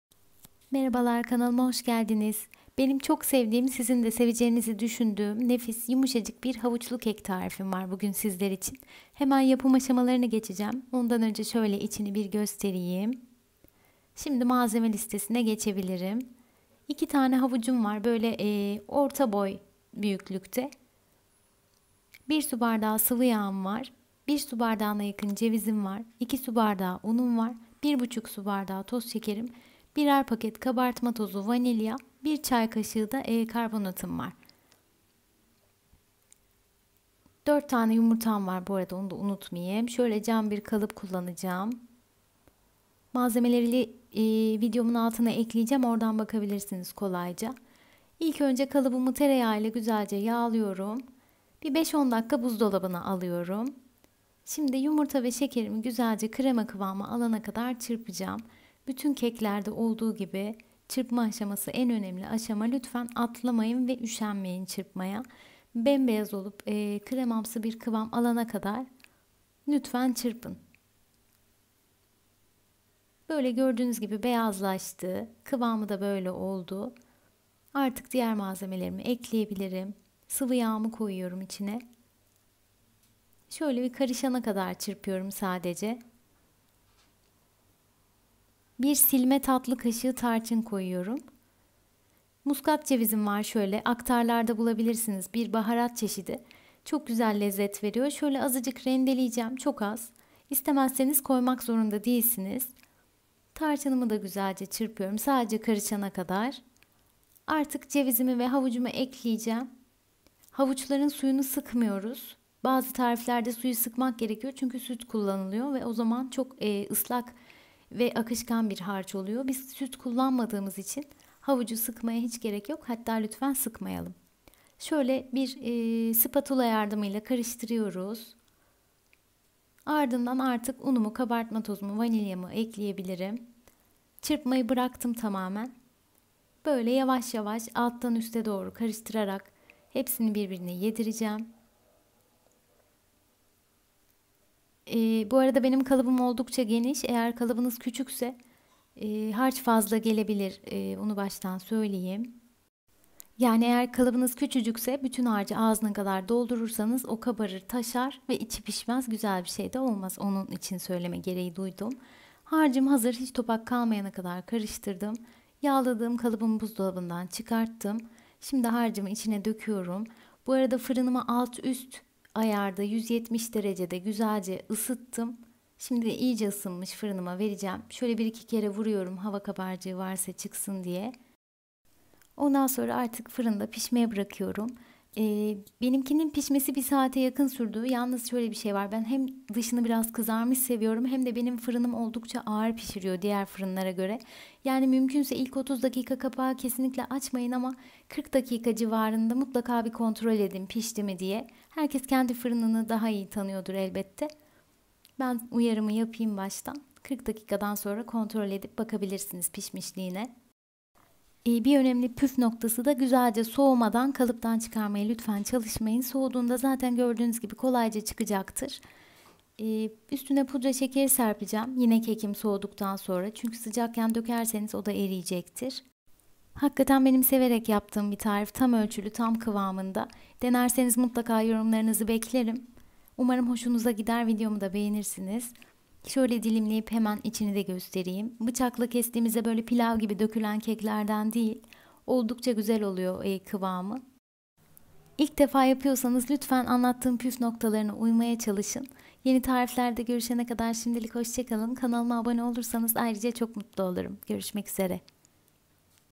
Havuçlu kek tarifi 🥕 Ceviz ve tarçınlı, tam ölçülü :)
malzemeler
2 adet havuc
1 su bardağı siviyag
1 su bardagi ceviz
2 su bardagi un
1,5 su bardagi toz seker
1 paket vanilya ve kabartma tozu
1 cay kasigi karbonat(sart degil)
1 silme tatli kasigi tarcin
4 yumurta
Varsa ve kullanmak isterseniz biraz da muskat rendeleyebilirsiniz.
170 derece onceden isinmis firinda
Beni instagramda da takip edebilirsinizz 👇🏻